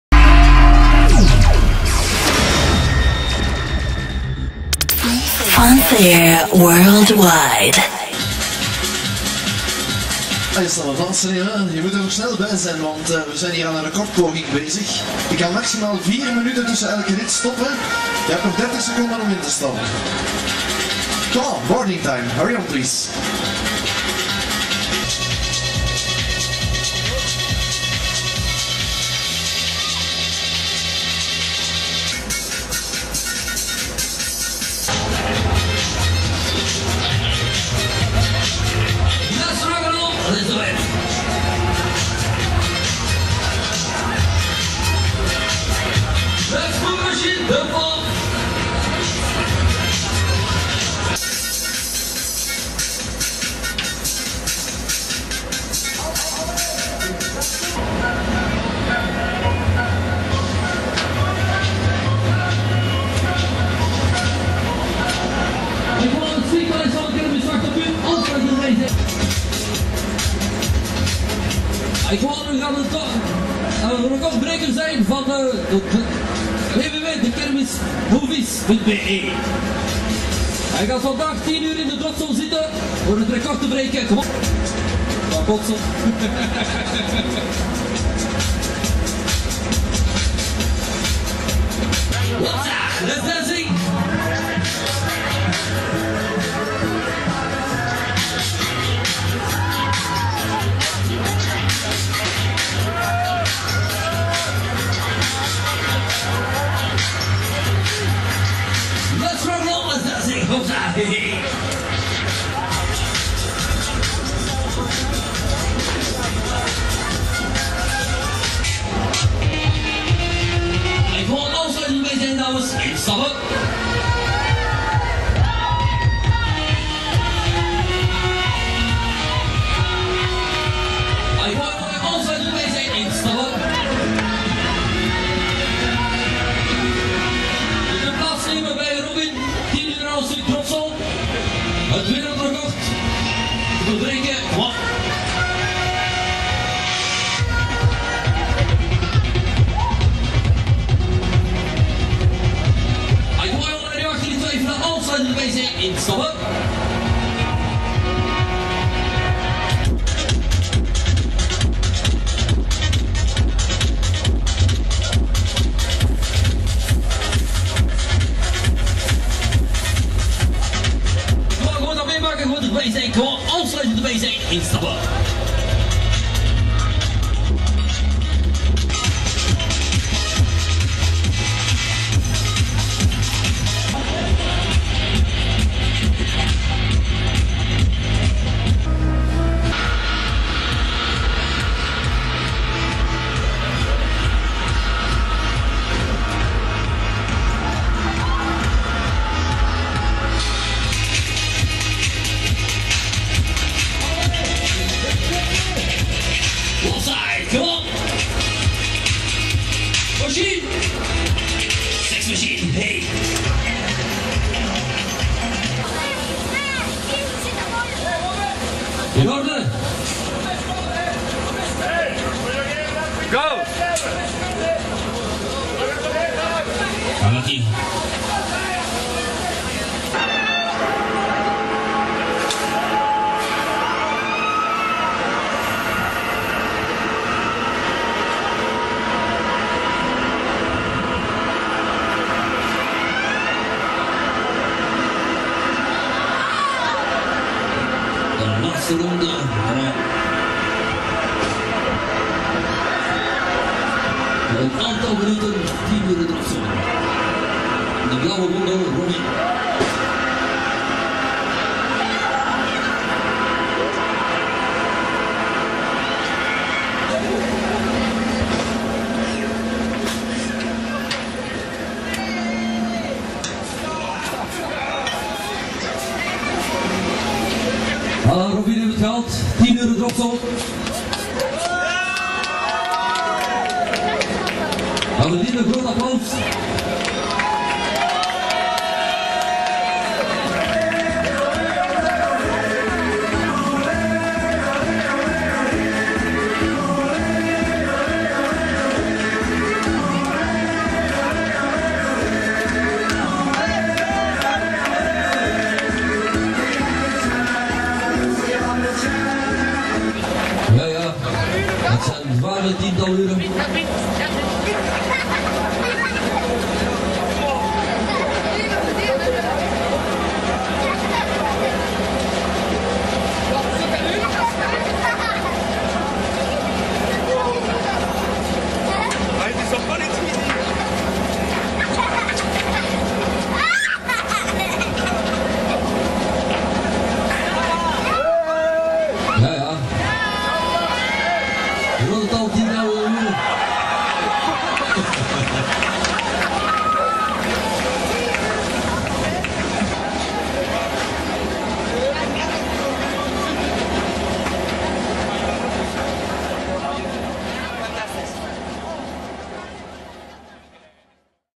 10 Hours "DROP ZONE" (Van Herck) @ Sinksenfoor Antwerpen (Belgium) 2007
Builder: Mondial
Type: Space roller / Top Scan
Owner: Van Herck Danny

Thanks to Loïck Capriolo for delivering my old kermismovies.be movies